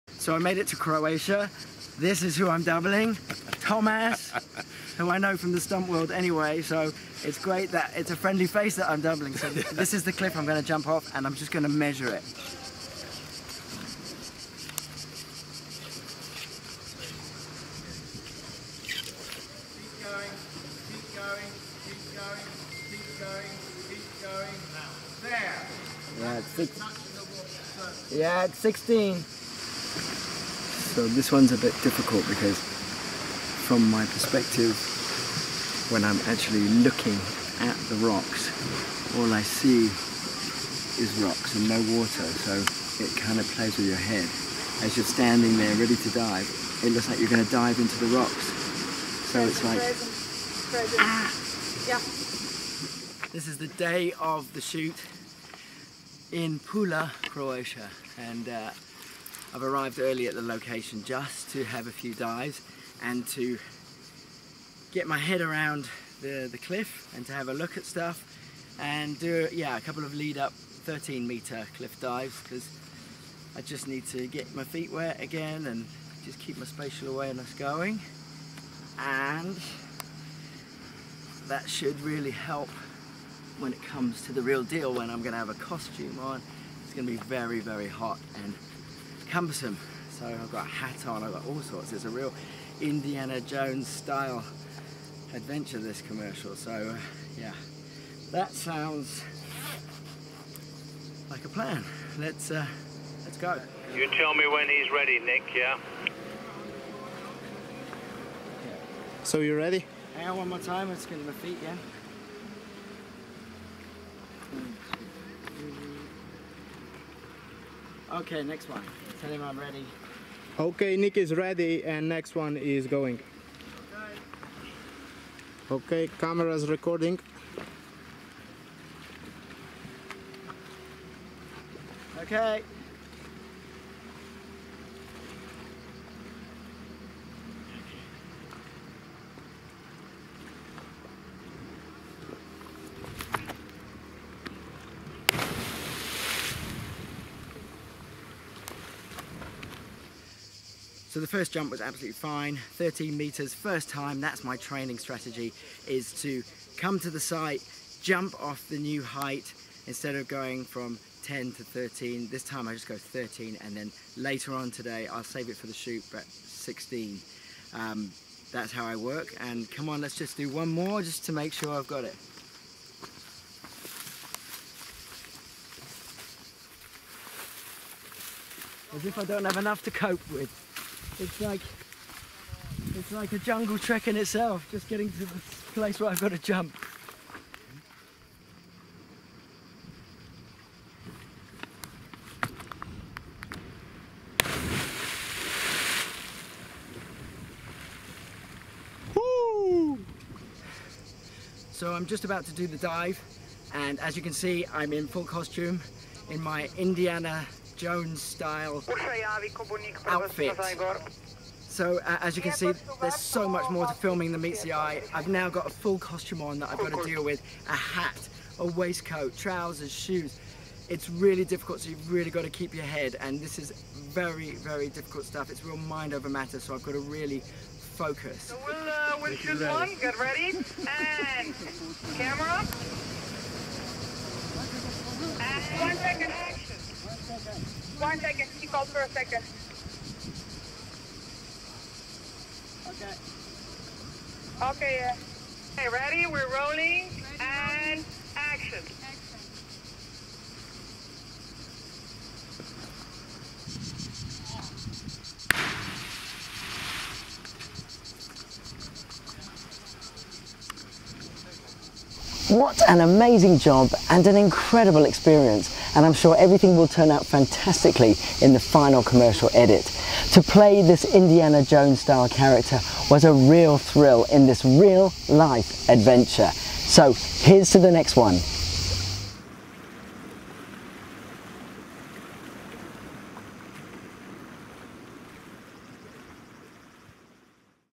Extreme Cliff Dive for TV Commercial
Cliff Diving for a TV Commercial in Croatia. A behind the scenes exclusive to show you just what goes into bringing this Indiana Jones adventure to Life